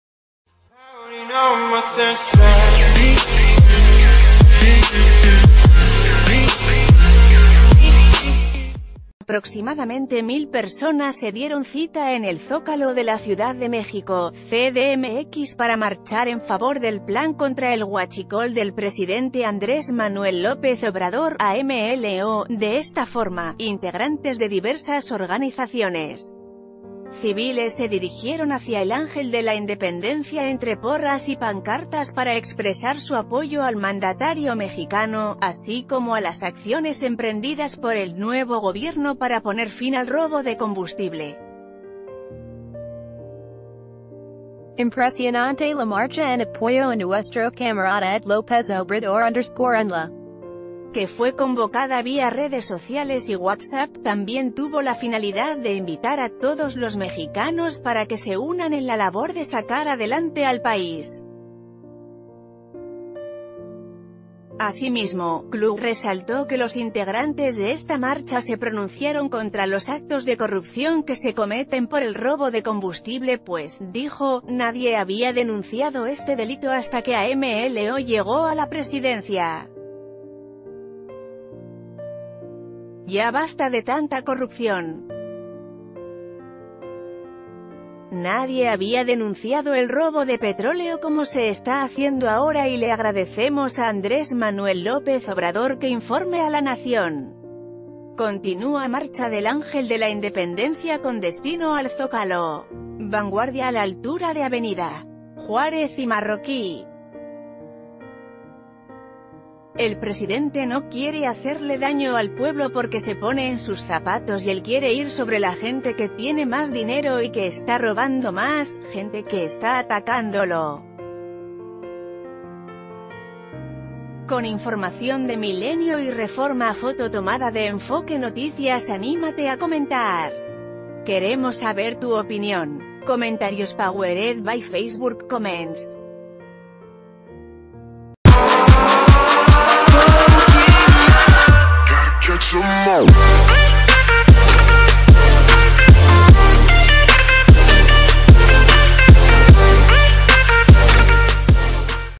Mexicanos marchan en CDMX en favor de AMLO y su plan antihuachicol
Aproximadamente mil personas se dieron cita en el Zócalo de la Ciudad de México (CDMX) para marchar en favor del plan contra el huachicol del presidente Andrés Manuel López Obrador (AMLO), De esta forma, integrantes de diversas organizaciones civiles se dirigieron hacia el Ángel de la Independencia ...